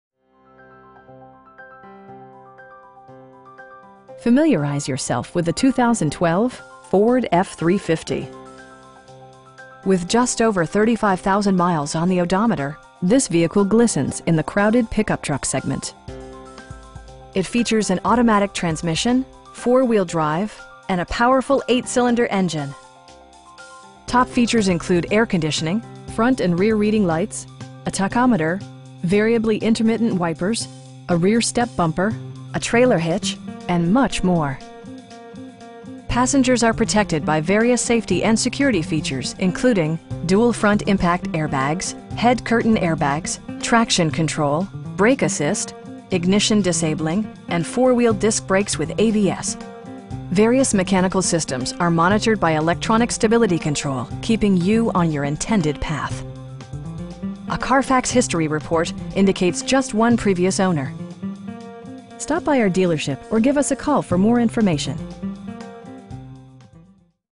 2012 Ford Super Duty F-350 SRW 4X4 Lariat
Gary Yeomans Ford LINCOLN
1420 N. Tomoka Farms Rd.

Familiarize yourself with the 2012 Ford F-350. With just over 35,000 miles on the odometer, this vehicle glistens in the crowded pickup truck segment! It features an automatic transmission, 4-wheel drive, and a powerful 8 cylinder engine. Top features include air conditioning, front and rear reading lights, a tachometer, variably intermittent wipers, a rear step bumper, telescoping steering wheel, a trailer hitch, and much more. Passengers are protected by various safety and security features, including: dual front impact airbags, head curtain airbags, traction control, brake assist, ignition disabling, and 4 wheel disc brakes with ABS. Various mechanical systems are monitored by electronic stability control, keeping you on your intended path. A Carfax history report indicates just one previous owner. We pride ourselves in consistently exceeding our customer's expectations. Stop by our dealership or give us a call for more information.